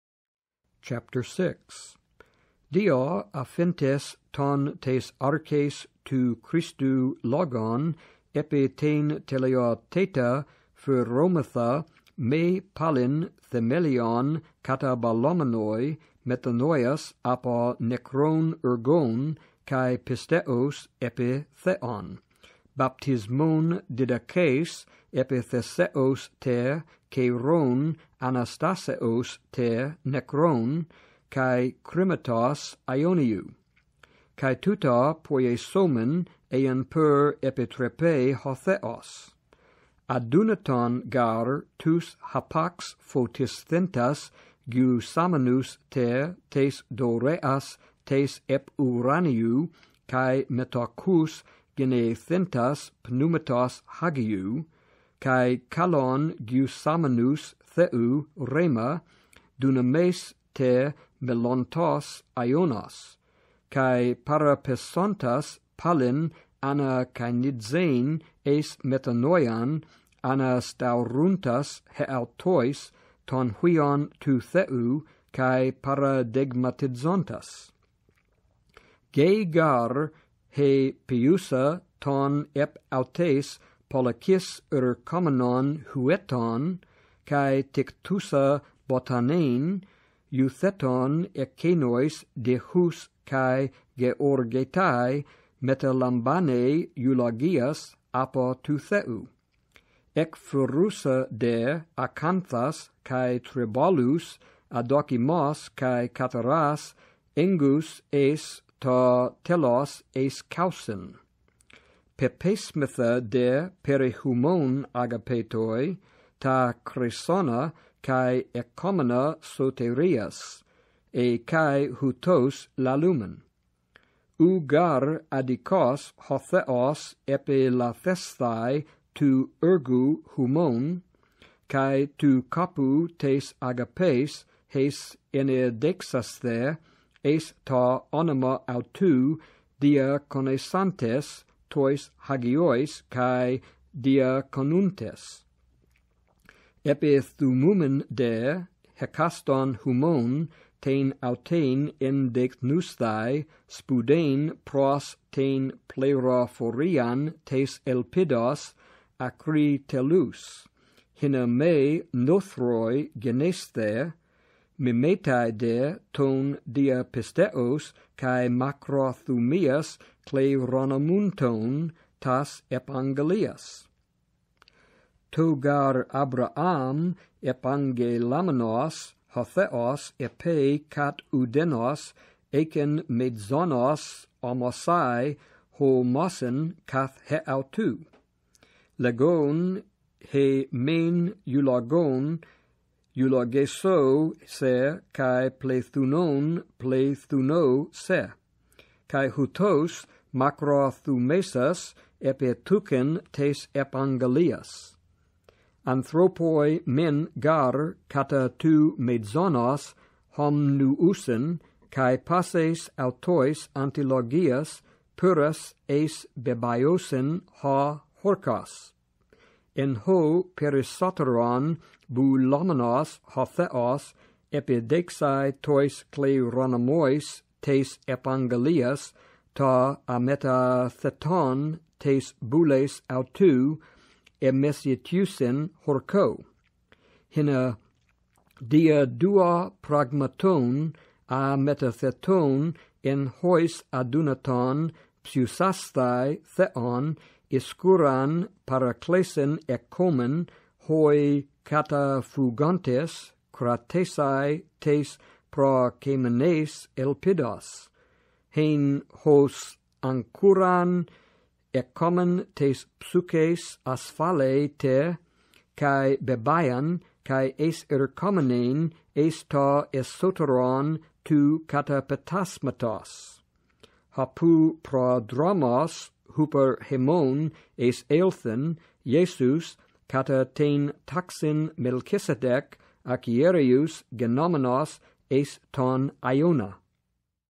Hebrews 6 in Greek (Maurice Robinson)
Maurice Robinson reeds Hebrews 6 in Greek (Robinson-Pierpont 2005 edition).